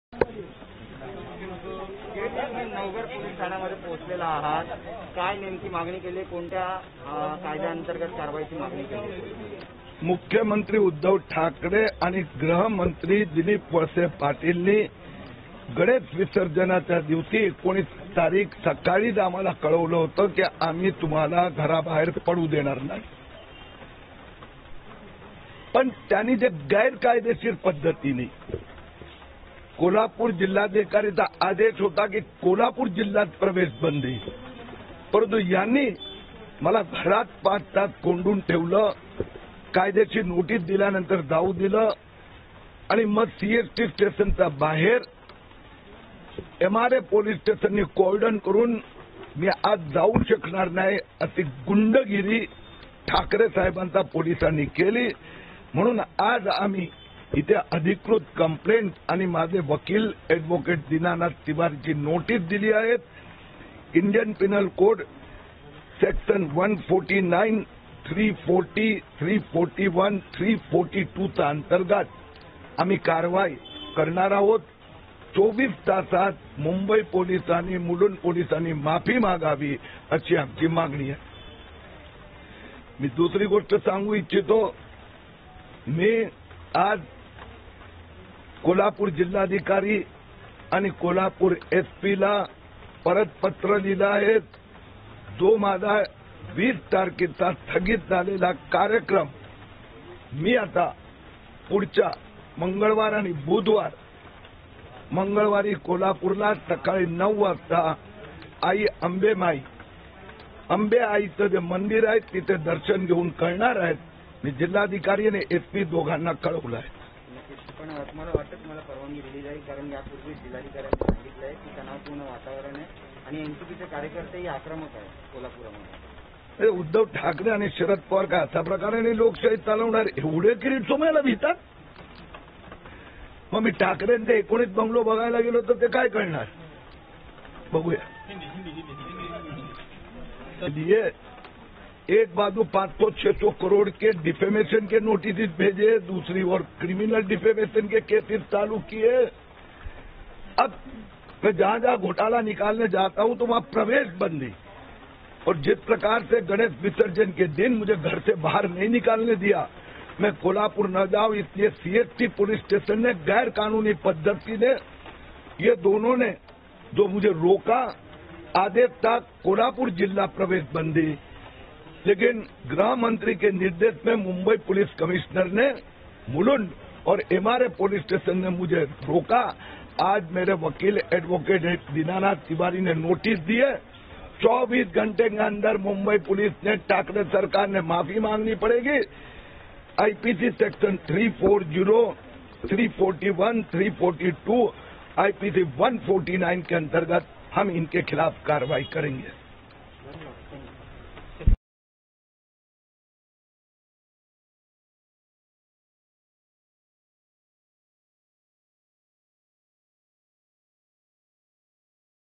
mumbai police should apologize or face action kirit somaiya lodged a complaint
किरीट सोमैया यांची मुलुंड पोलीस ठाण्यांमध्ये मुलुंड पोलीस ठाण्याचे वरिष्ठ पोलीस निरीक्षक आणि एसीपी त्यांच्या विरोधात तक्रार दाखल झाली आहे. 19 सप्टेंबरला मुंबई पोलिसांनी मला बेकायदेशीररित्या घरामध्ये सहा तास स्थानबद्ध करून ठेवले होते, असा आरोप सोमैयांनी केला आहे. तसेच मुंबई पोलिसांनी 24 तासांच्या आत माझी माफी मागावी, अन्यथा इतर कायदेशीर कारवाईला सामोरे जावे, असे सोमैया यांनी म्हटले आहे.